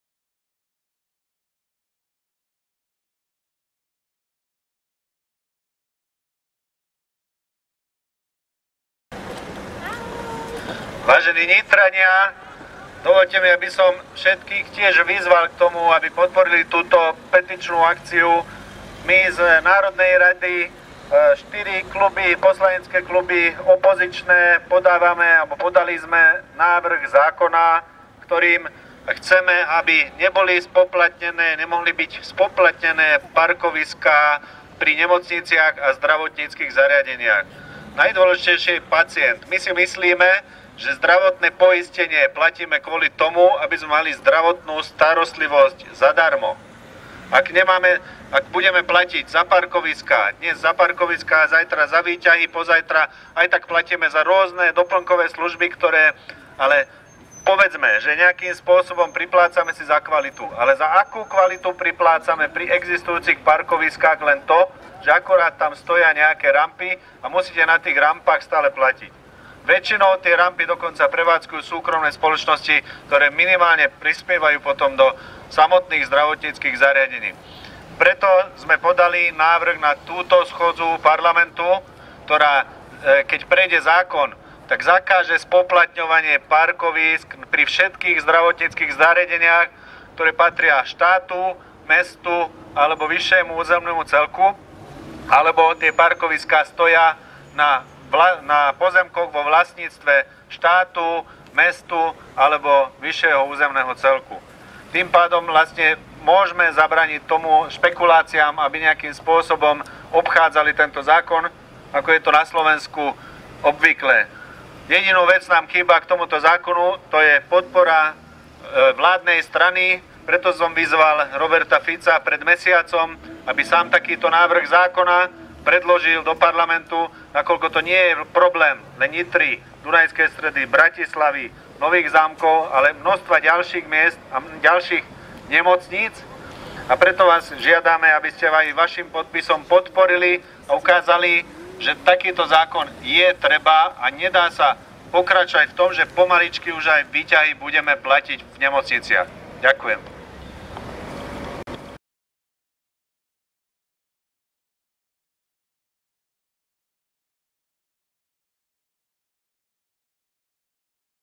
József Nagy na protestnom zhromaždení pred nemocnicou v Nitre
Občianski aktivisti v rámci série protestných zhromaždení  proti spoplatneniu parkovísk pred nemocnicami a zdravotníckymi zariadeniami sa v stredu 12. marca 2014 zišli v ďalšom slovenskom meste - tentoraz v Nitre. Podporiť ich prišiel aj poslanec NR SR za Most-Híd, József Nagy. Nagy predstavil ľuďom návrh zákona, ktorý by umožnil pacientom pred nemocnicami a zdravotníckymi zariadeniami parkovať zdarma. „Nechceme, aby to dopadlo tak, že občania si budú platiť v nemocniciach pomaly aj za používanie výťahov," povedal József Nagy.